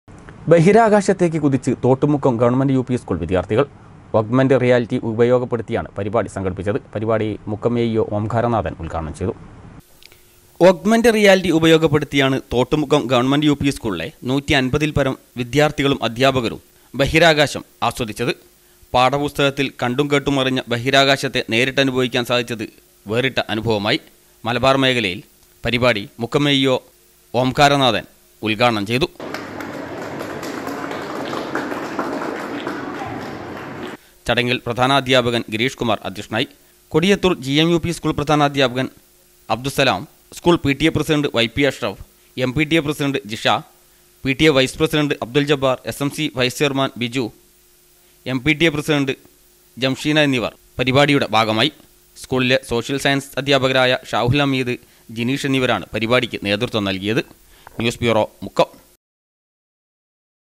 ബഹിരാകാശത്തേക്ക് കുതിച്ച് തോട്ടുമുക്കം ഗവ: യു പി സ്കൂൾ വിദ്യാർത്ഥികൾ
ബഹിരാകാശത്തേക്ക് കുതിച്ച് തോട്ടുമുക്കം ഗവ: യു പി സ്കൂൾ വിദ്യാർത്ഥികൾ.ഒഗ്മെൻ്റ് റിയാലിറ്റി ഉപയോഗപ്പെടുത്തിയാണ് പരിപാടി സംഘടിപ്പിച്ചത്.